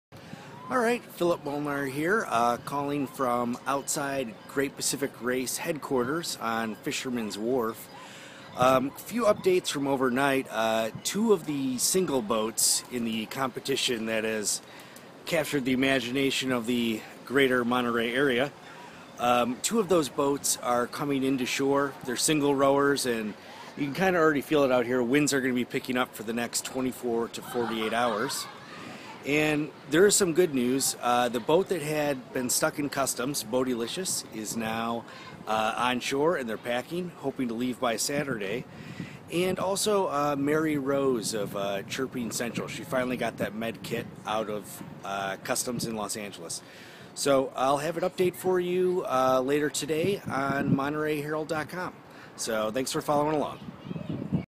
Molnar here. Thursday morning. Land report on GreatPacificRace from @cityofmonterey Fisherman's Wha
Molnar here. Thursday morning. Land report on GreatPacificRace from @cityofmonterey Fisherman's Wharf No.2.

By: Phil Molnar
Published on: June 12, 2014